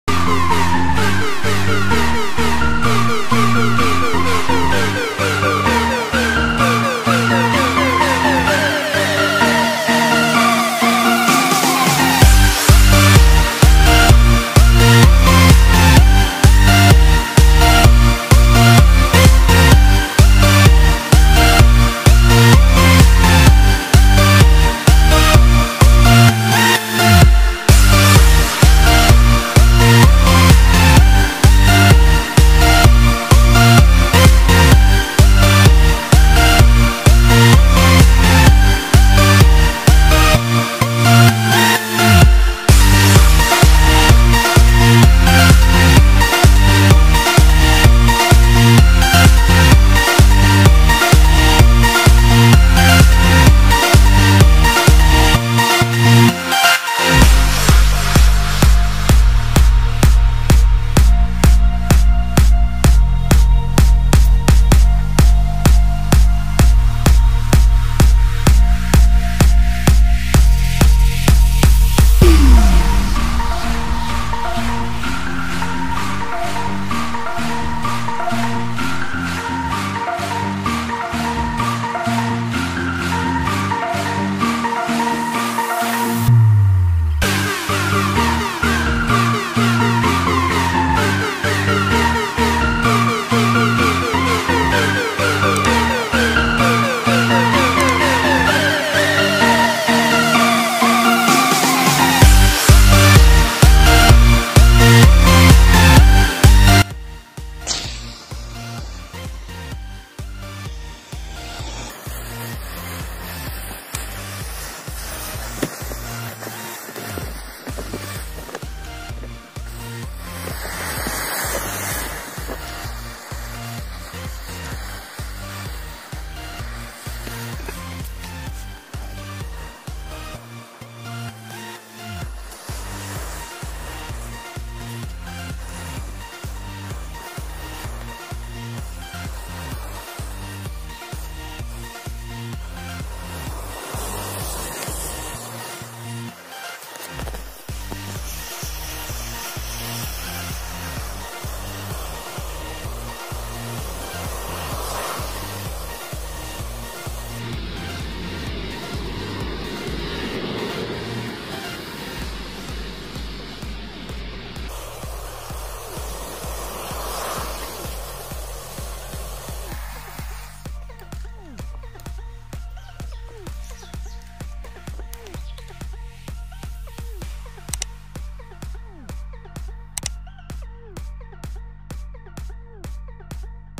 Fire Domino VOLCANO ERUPTION Awesome Match Chain Reaction Don t Try At Home
Hi, In this experiment matches, we burn a volcano from about 50,000 matches. This volcano make a lot of time and effort. Don't try it in the house.